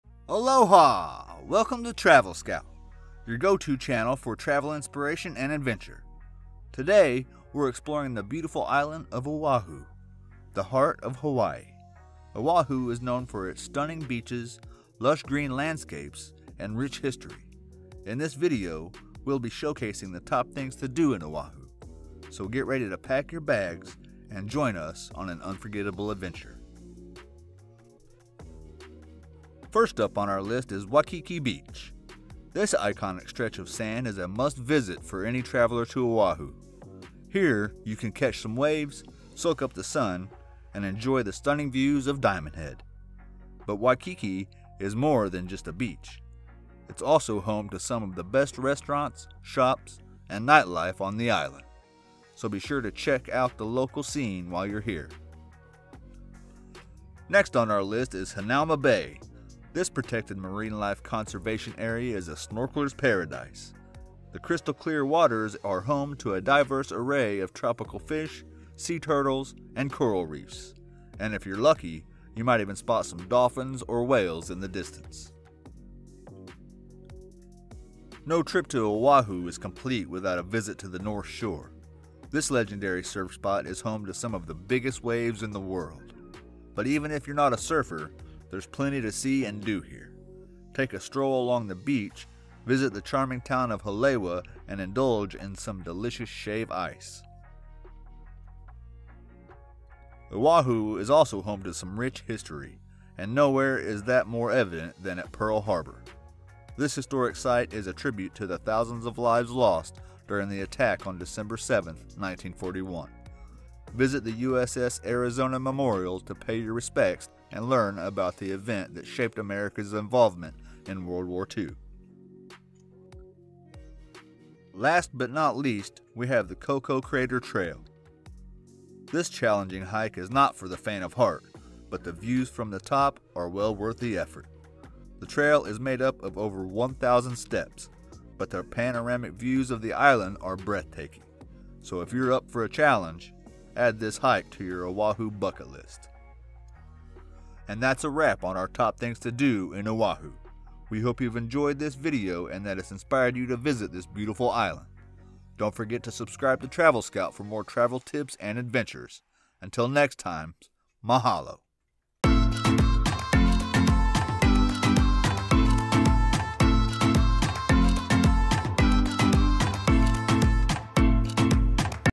Top Things To Do in Oahu Hawaii
If you're looking for things to do in Oahu, then you've come to the right place! In this video, we'll share with you some of the top activities you can do in Oahu, Hawaii.

From hiking to shopping to kayaking, we've got everything you need to enjoy your time in Oahu. Whether you're a first-time visitor or an experienced traveler, there's something for you in this video!

Looking for things to do in Oahu? Look no further! In this video, we'll share with you our top picks for the best things to do in Oahu. From the best beaches to the coolest shopping districts, we'll have you covered!

We hope you enjoy our Top Things To Do in Oahu video. If you have any questions or comments, please let us know!